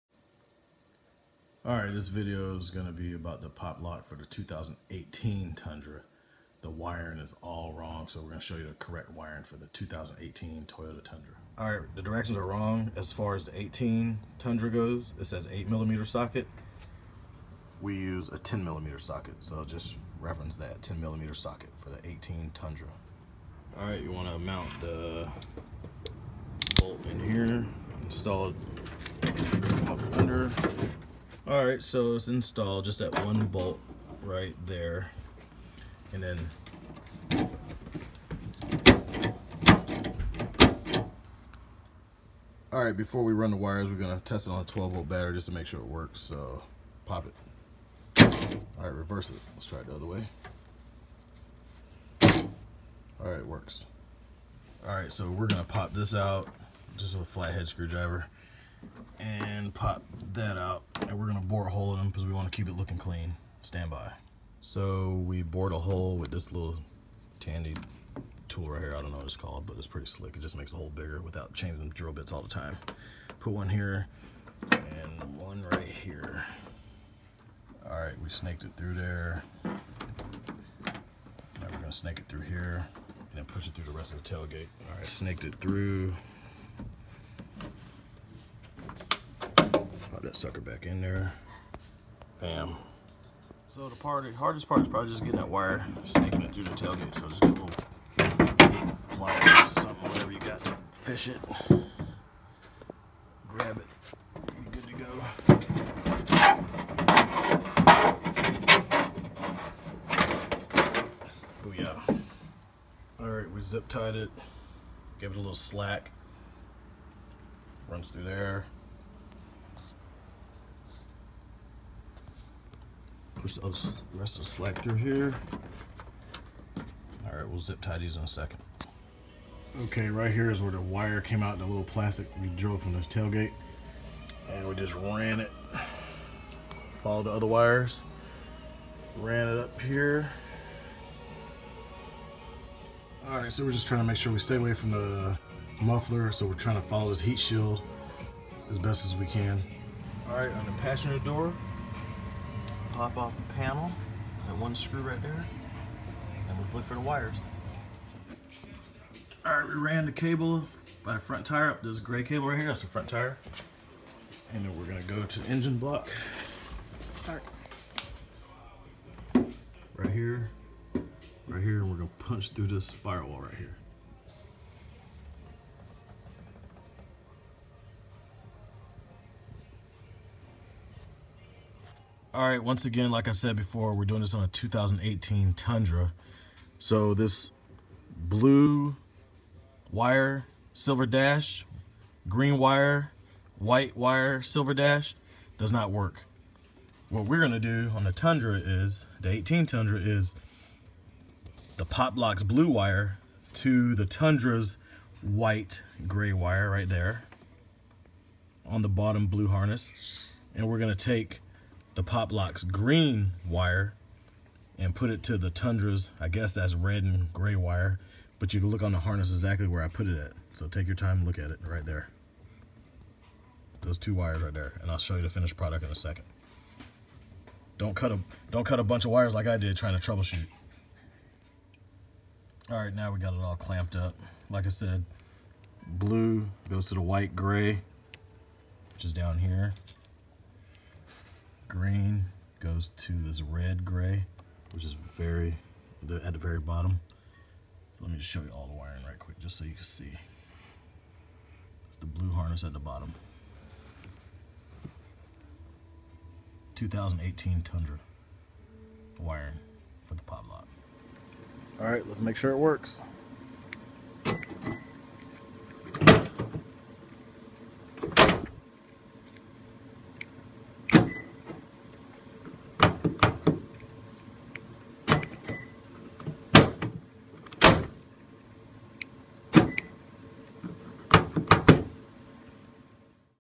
2018 Toyota Tundra Tailgate Pop & Lock Install w/Correct Wiring
How to install the Pop Lock for your tailgate on a 2018 Toyota Tundra.  Lock and Unlock your tailgate with your factory remote now!